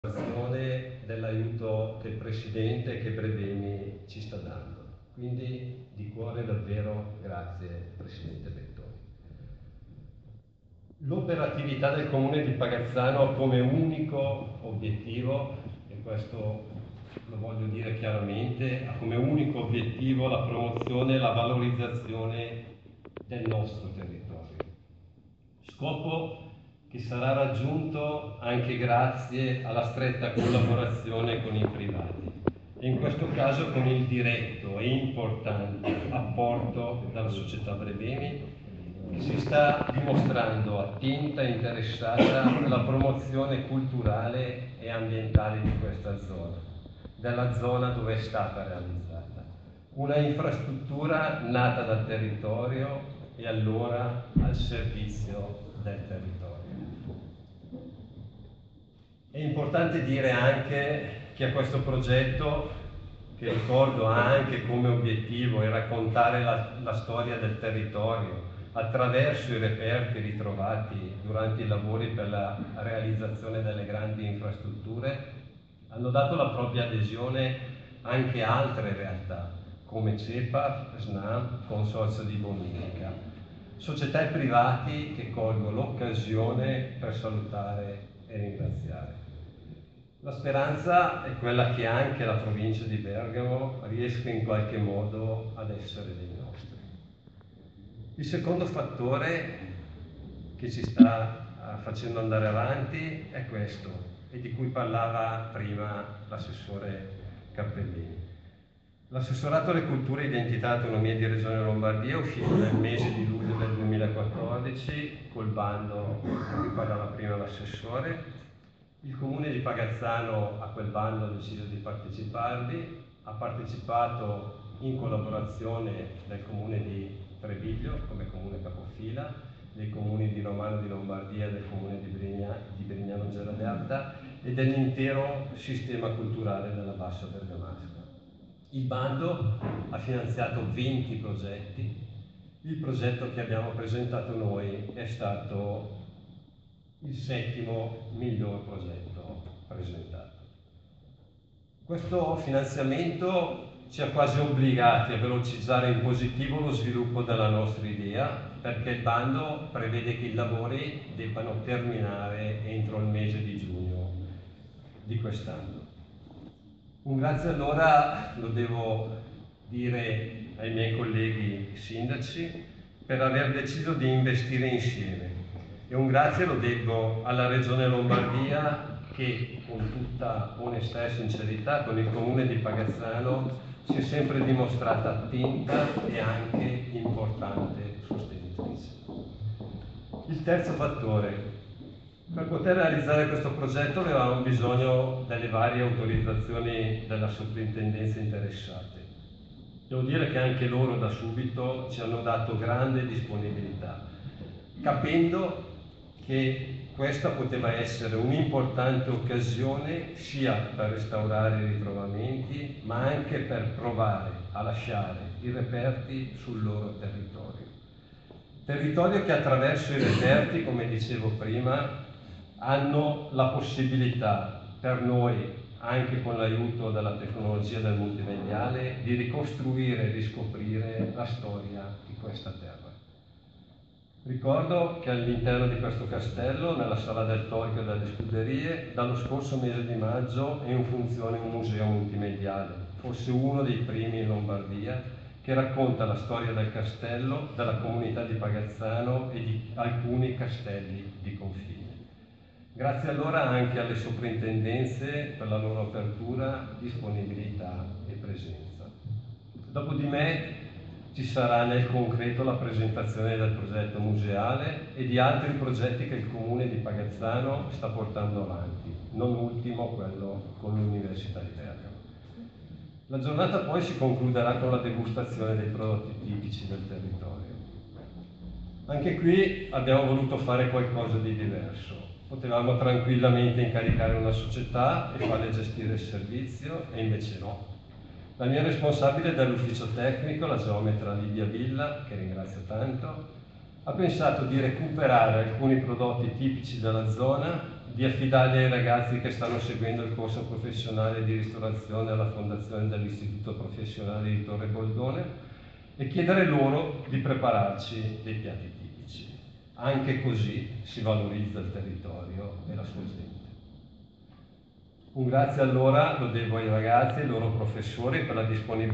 Raffaele Moriggi, sindaco di Pagazzano
Grandi Opere, Grandi Scoperte.
I reperti orcheologici roccontono il territorio

Tavola Rotonda
I tesori di Brebemi, dalla scoperta alla musealizzazione
14 febbraio 2015, Castello Visconteo di Pagazzano
Recorded with my Android Mobile Phone.